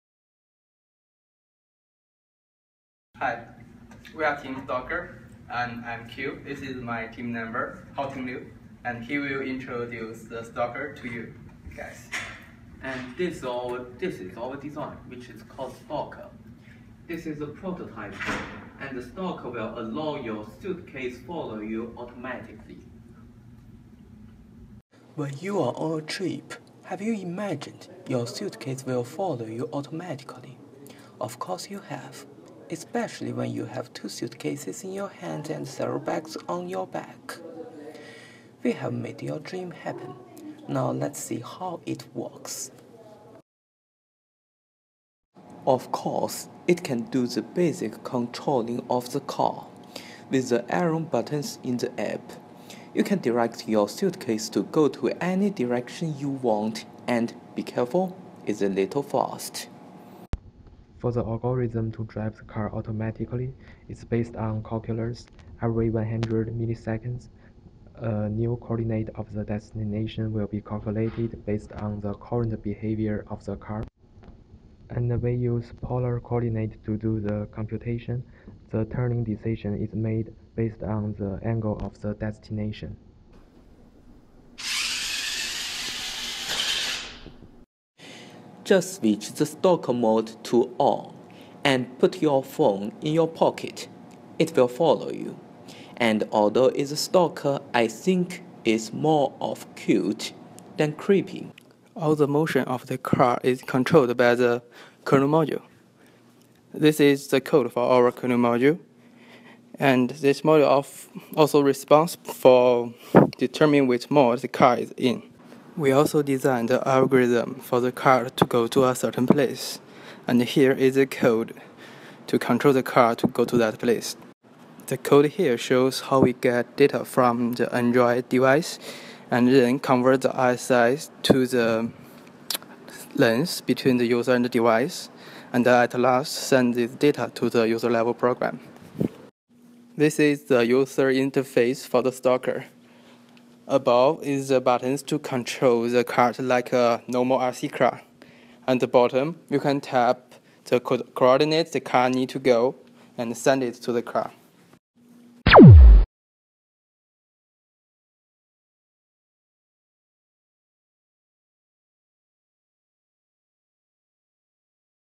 EC535 Final Project：Stalker-suitcase (you can put a camera on it,~）
It is the video of the final project of EC535 whose name is stalker. It can follow its user automatically.
Don't ask me why should you put a camera on it, I won't tell you~